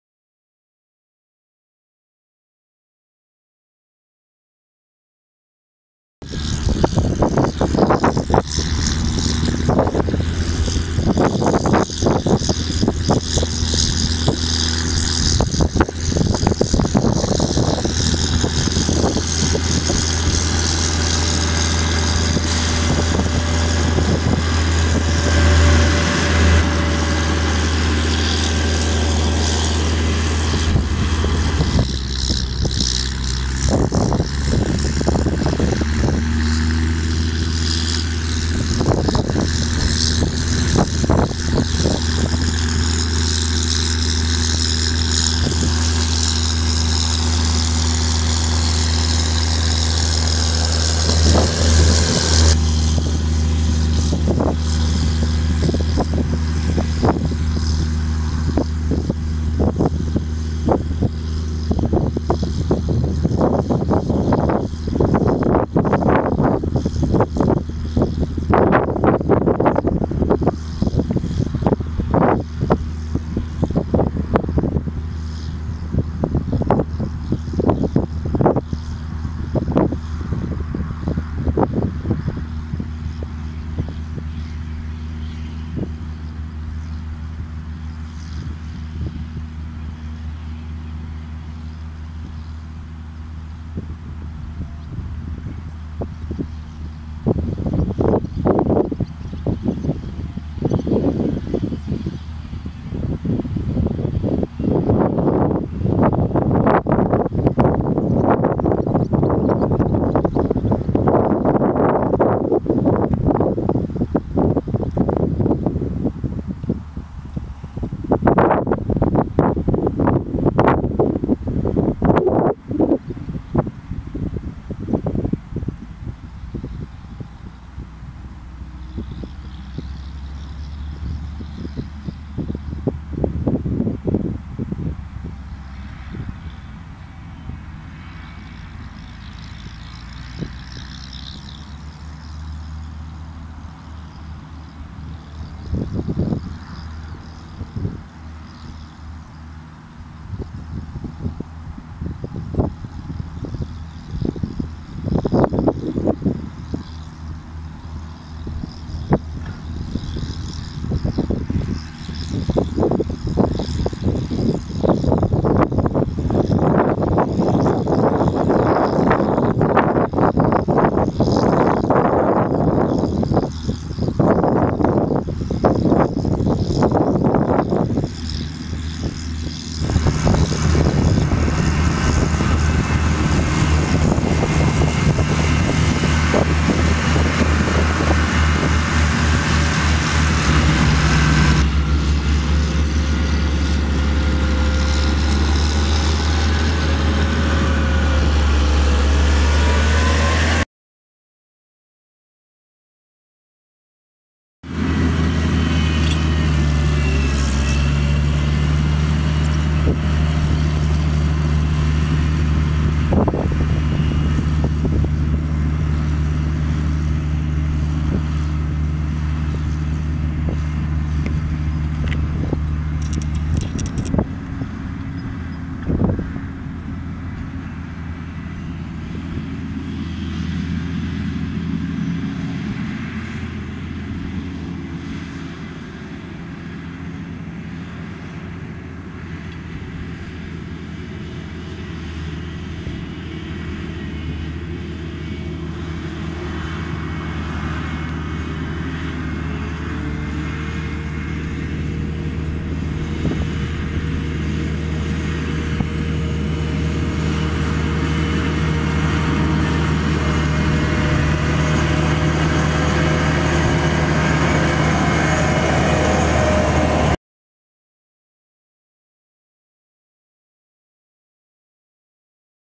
loonbedrijf prenger injecteren challenger terre gator verveat hydro trac
loonbedrijf prenger aan het injecteren op de laatste dag toen er nog geinjecteert mocht worden. Met 2 dikke combinaties om snel de laatste mest in de grond te krijgen
like mijn facebook pagina om de gehele reportage`s van de foto`s die voor komen in de video